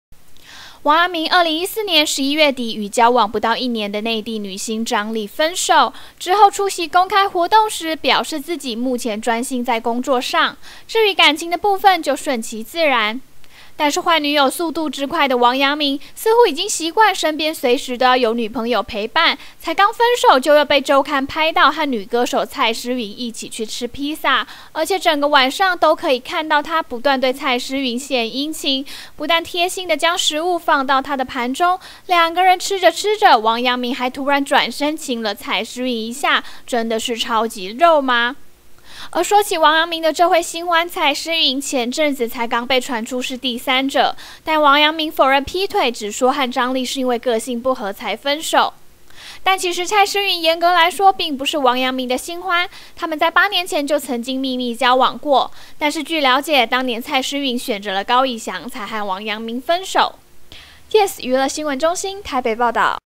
【5年前】王陽明回收舊愛 吃飯中轉身親蔡詩蕓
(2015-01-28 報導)

王陽明2014年11月底與交往不到一年的內地女星張儷分手，之後出席公開活動時 表示自己目前專心在工作上，至於感情的部分就順其自然。但是換女友速度之快的王陽明，似乎已經習慣身邊隨時都要有女朋友陪伴，才剛分手就又被週刊拍到和女歌手蔡詩蕓一起去吃披薩。